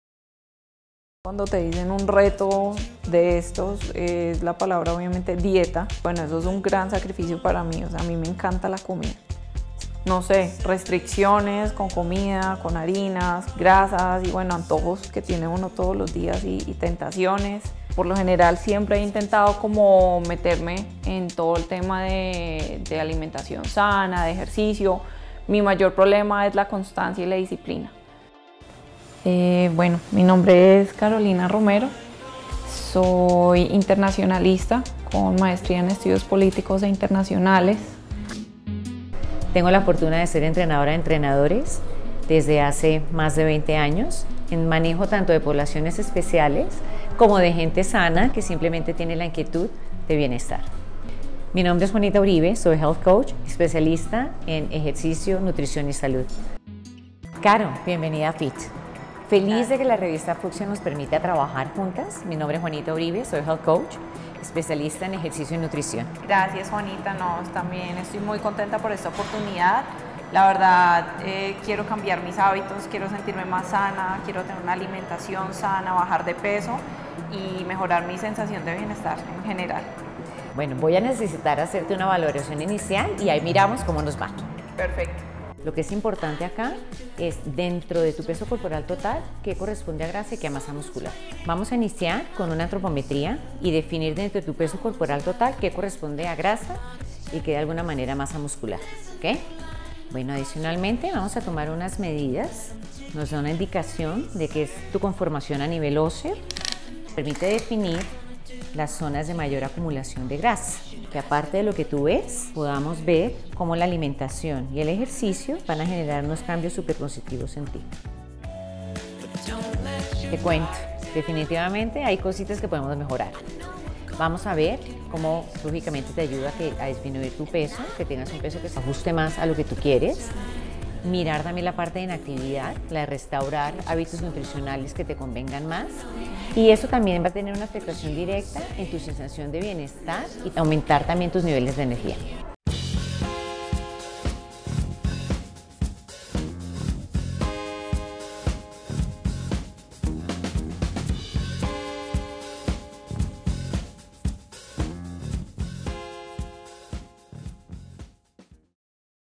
Capítulo 1 ¿Te unes al reto MujeresFirmes?
Sigue a Carolina Romero en su búsqueda de un estilo de vida más saludable